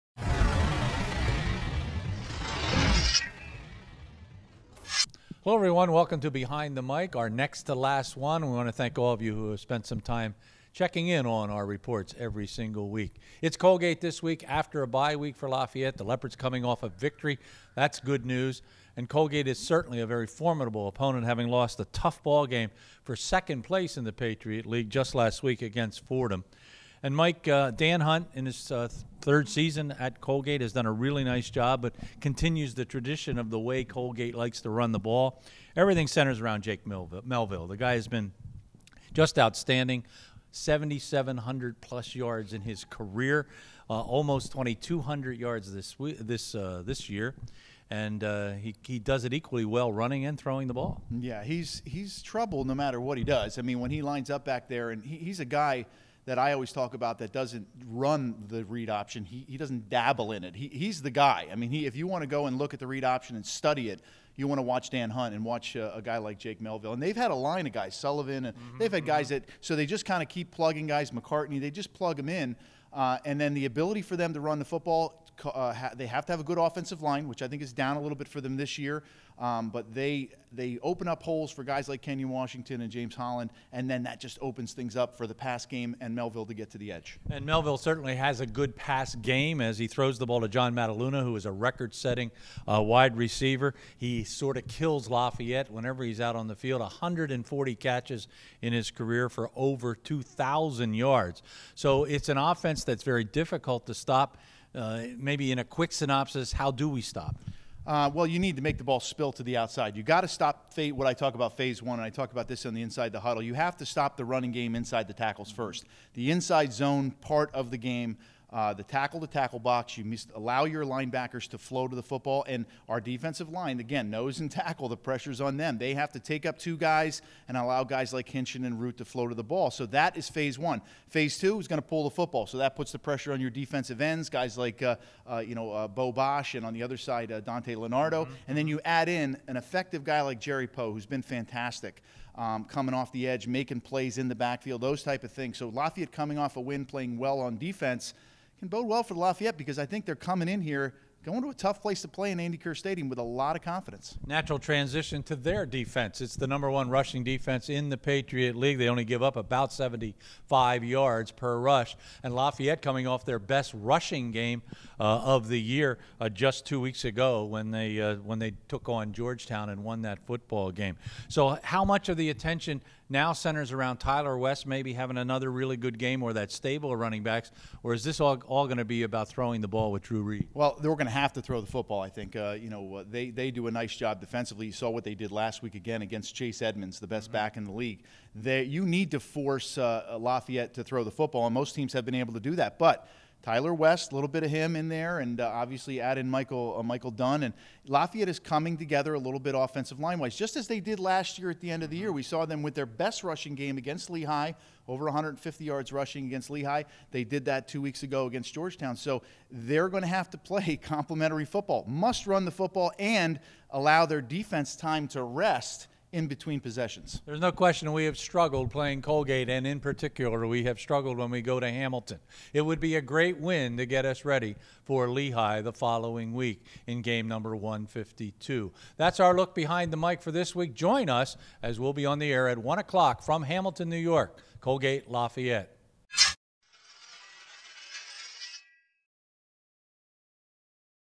Behind the Mic: Lafayette Football at Colgate
LSN's Gary Laubach and Mike Joseph go Behind the Mic to discuss Lafayette's next matchup against the Colgate Raiders.